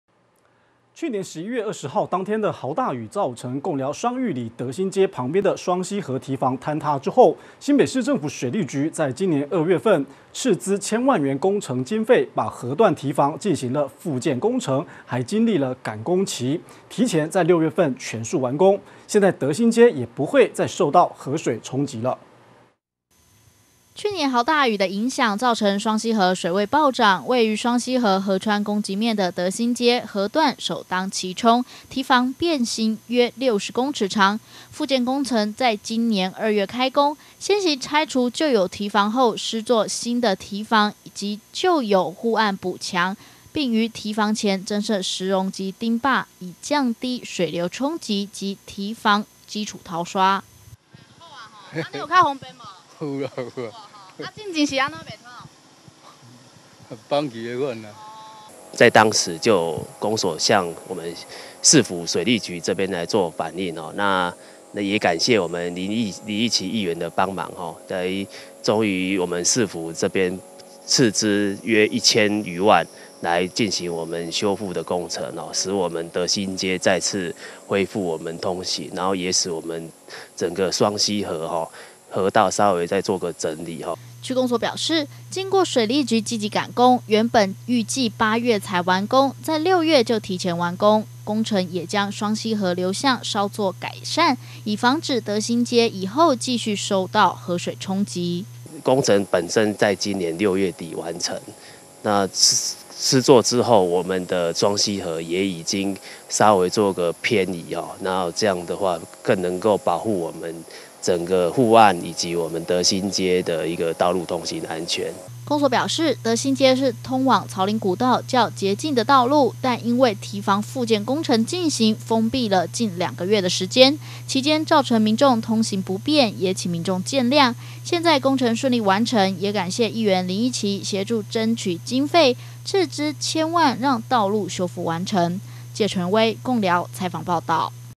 1050701觀天下新聞HD03 貢寮區德心街路基掏空 斥資千萬整修完成
歷經去年11月20日豪大雨造成貢寮區雙玉里德心街旁的雙溪河堤防坍塌，新北市政府水利局於今年2月，斥資千萬將河段堤防復建工程並加速趕工，提前於今年6月份完工，也讓民眾終於可以有路可以走。